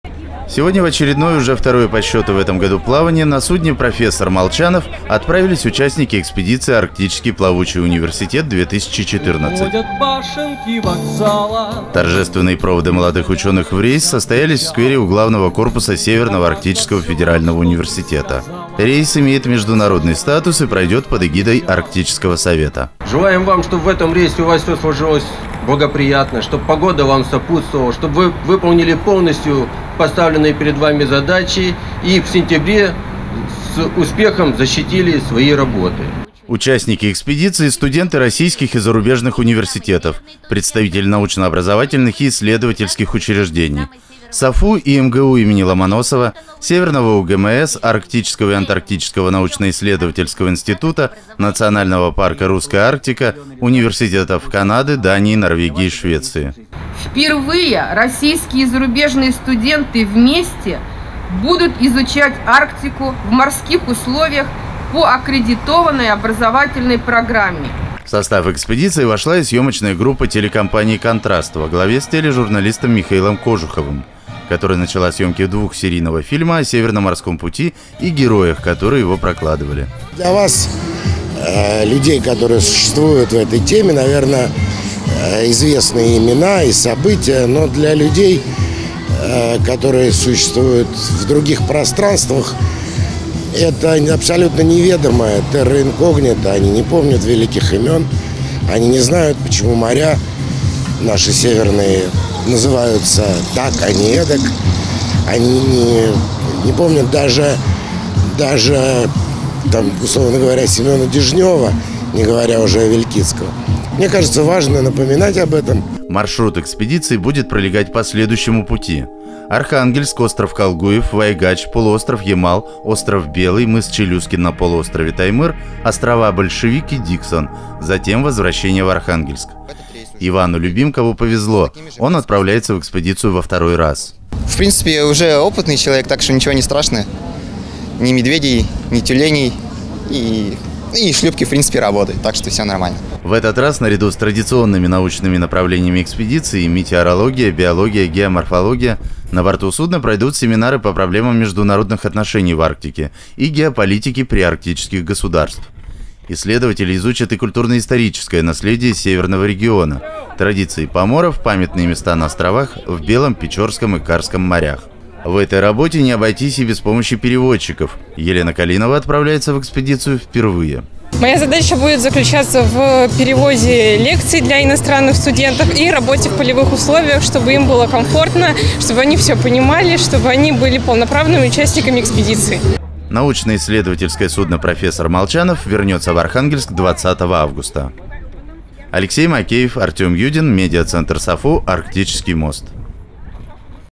«Арктический плавучий университет» отправился во второй рейс 2014 года
Сегодня на судне «Профессор Молчанов» в северные широты отправились участники второго рейса экспедиции «Арктический плавучий университет — 2014».

Это уже шестая экспедиция по совместному проекту САФУ и Северного УГМС, поддержанному Русским географическим обществом. Проект АПУ стартовал в 2012 году.

В этот раз международный экспедиционный отряд состоит из 55 человек. Участники экспедиции — студенты российских и зарубежных университетов, представители научно-образовательных и исследовательских учреждений: Северный (Арктический) федеральный университет имени М.В. Ломоносова, МГУ имени М.В. Ломоносова, Северное УГМС, Арктический и антарктический научно — исследовательский институт, НЦ «Русская Арктика», заповедник «Вишенский», Ассоциация полярников РФ, Университет Северной Британской Колумбии (Канада), Университет Рокшильде (Дания), Университет Копенгагена (Дания), Университет Тромсе (Норвегия), Университет Лулео (Швеция), компания Total (Франция). В состав экспедиции вошла и съемочная группа телекомпании «Контраст» во главе с генеральным директором, тележурналистом, Михаилом Кожуховым.

«Впервые российские и зарубежные студенты вместе будут изучать Арктику в морских условиях по аккредитованной образовательной программе. Впервые в истории проекта будут проведены работы по оценке состояния Северного морского пути, историко-культурные исследования по поиску и изучению объектов, связанных с освоением Севера», — подчеркнула особенности второй экспедиции 2014 года в своей напутственной речи Елена Кудряшова, ректор САФУ.

«Сегодня когорта исследователей Арктики пополнится молодыми именами. Желаю вам, чтобы в этом рейсе все у вас сложилось благоприятно, чтобы погода вам сопутствовала, чтобы вы полностью выполнили поставленные перед вами задачи и в сентябре успешно защитили свои работы», — поздравил участников экспедиции Александр Дрикер, заместитель начальника Северного УГМС. Он также отметил, что в этом году исполняется 180 лет со дня основания Гидрометслужбы России.

На борту НИС «Профессор Молчанов» состоялась пресс-конференция, где журналисты смогли пообщаться с руководителями САФУ и Северного УГМС, руководителем экспедиции, директором Арктического центра стратегических исследований Константином Зайковым и известным телеведущим Михаилом Кожуховым.

Маршрут экспедиции, проходящей под эгидой Арктического совета, будет пролегать по Северному морскому пути: Архангельск — п. Бугрино (о.Колгуев) — о. Вайгач — ст. Марресаля (п-ов Ямал) — ст. Попова (о. Белый) — м. Челюскин (п-ов Таймыр)— ст. Барановская (о. Большевик) — о. Диксон — Архангельск.